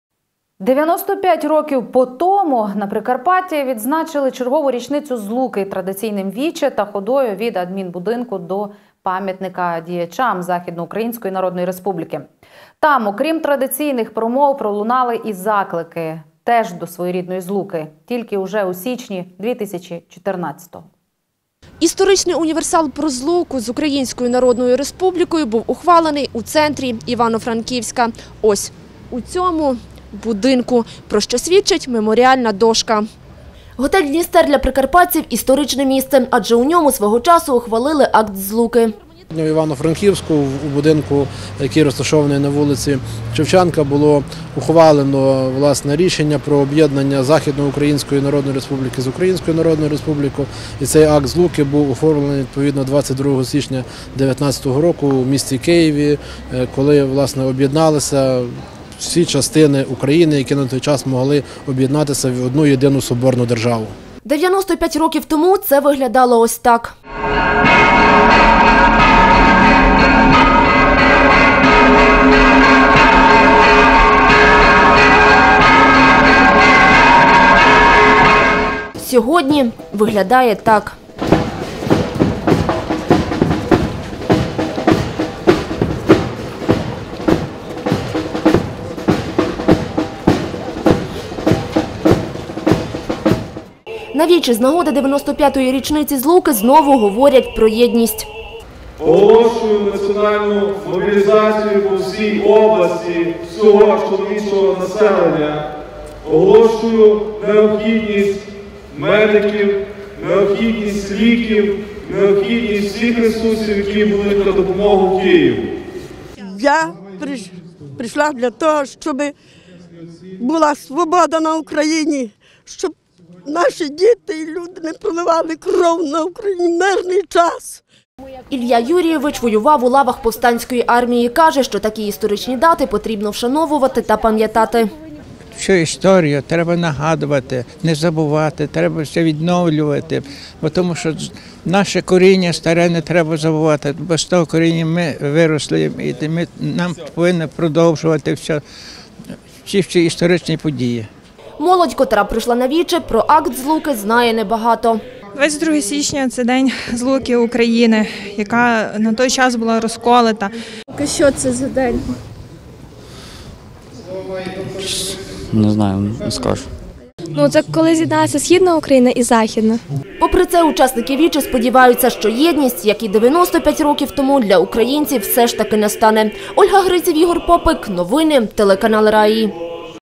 Чергову річницю Злуки традиційним віче та ходою від адмінбудинку до пам'ятника діячам ЗУНР
95 років по тому на Прикарпатті відзначили чергову річницю Злуки традиційним віче та ходою від адмінбудинку до пам'ятника діячам Західно-Української Народної Республіки. Там, окрім традиційних промов, пролунали і заклики. Теж до своєрідної Злуки. Тільки вже у січні 2014-го.